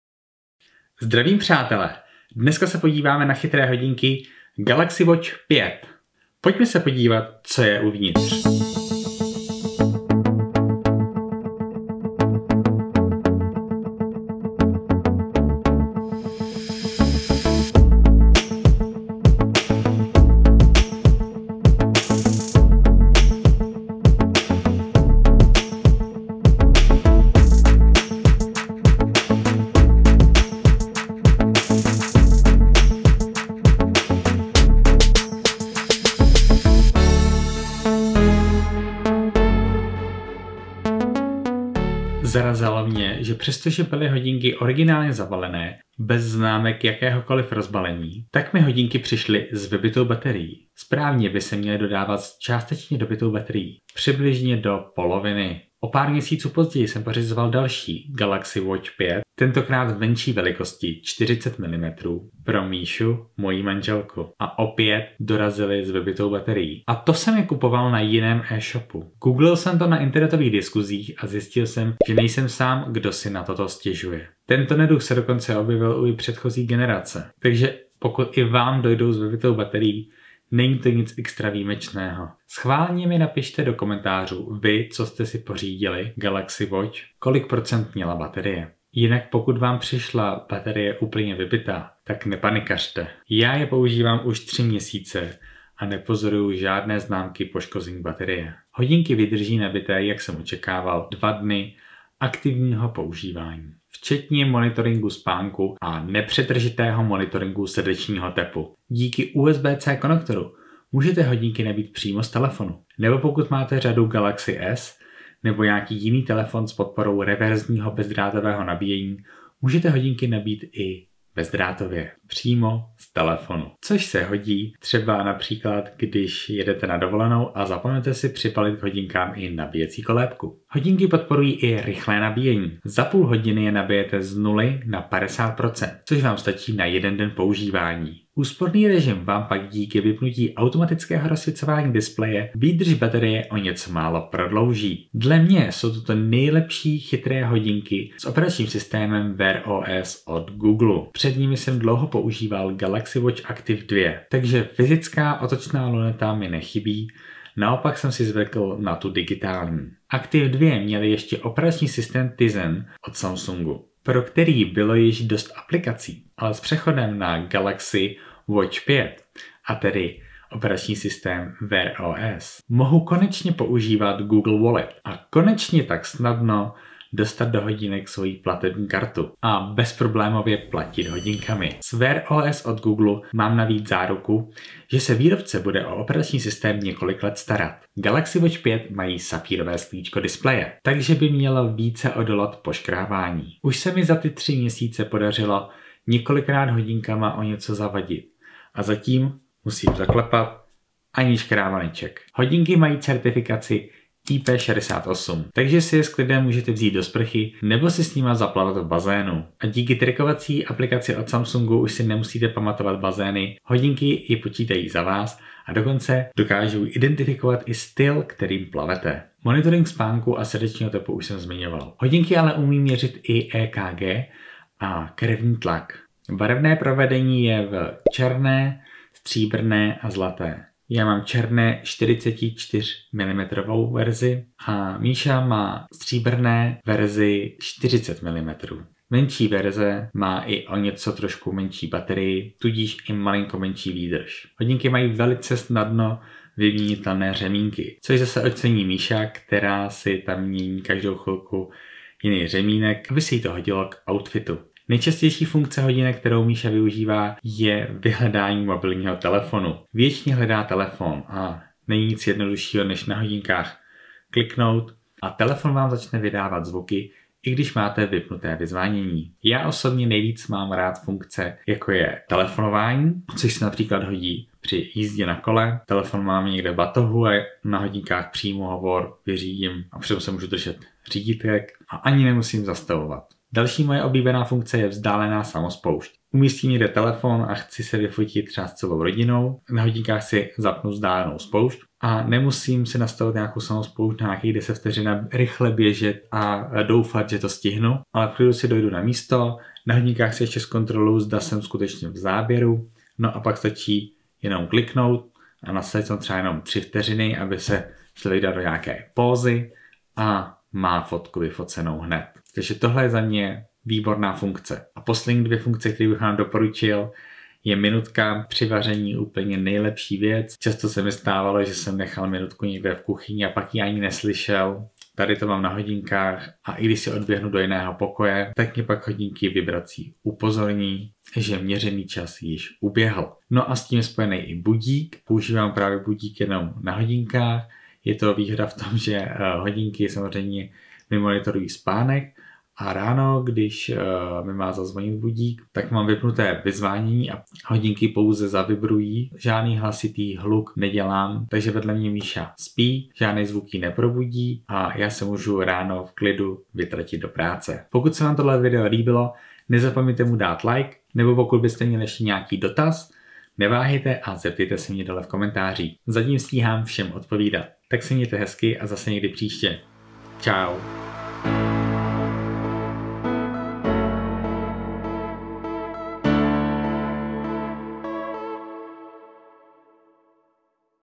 ⌚ Samsung Galaxy Watch 5 - Unboxing a recenze od Magoga
Unboxing a recenze na chytré hodinky Samsung Galaxy Watch 5